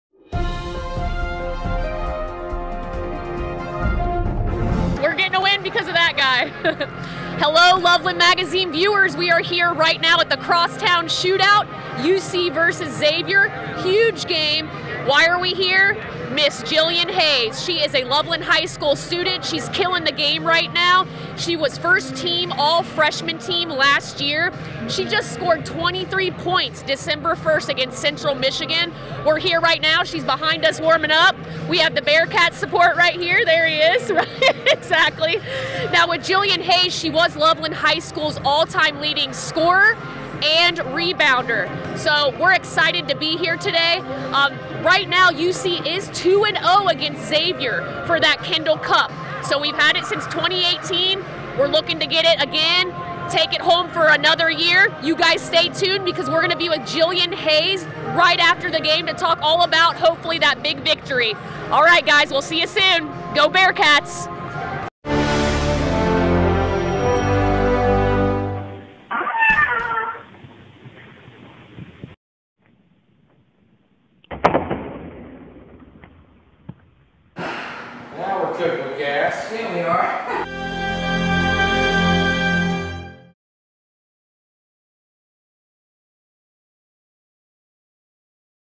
Cassie Mattia Talks About Jillian Hayes and the UC Bearcats before the Crosstown Shootout!
Join Cassie Mattia as she talks Bearcats and Jillian Hayes stats before the start of the 2021 Skyline Chili Crosstown Shootout!